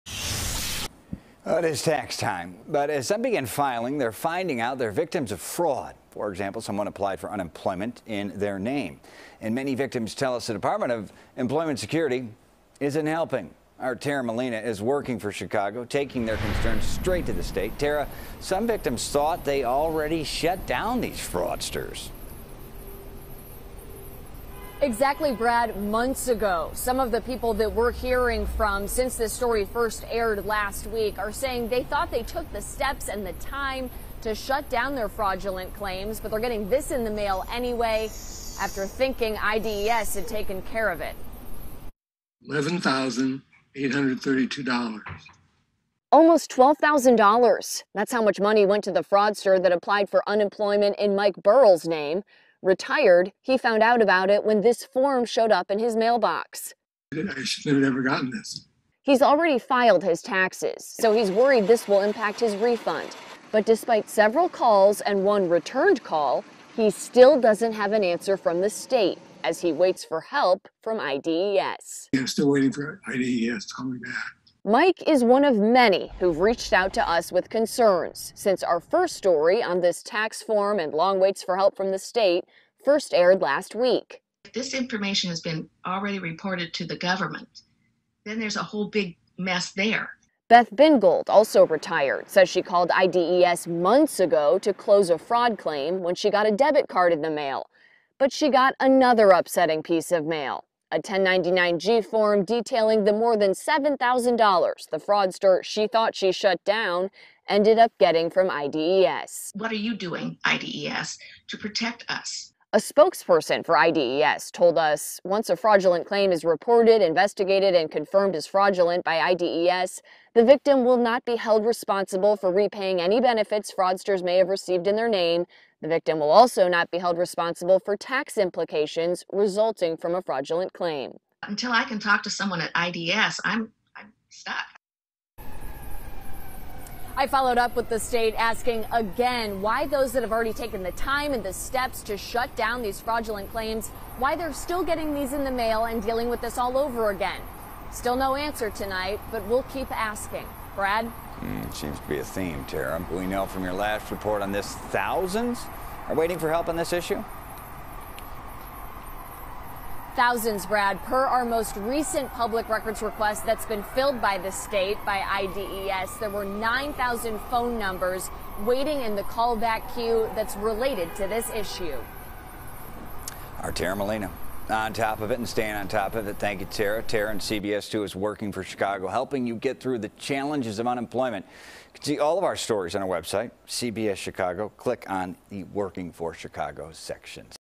Unemployment Fraud Victims Finding Out About Fraudulent Claims Through 1099-G Tax Forms
It’s tax time – and as some begin filing, they are finding out they're victims of fraud after someone applied for unemployment in their name. CBS 2's Tara Molina reports.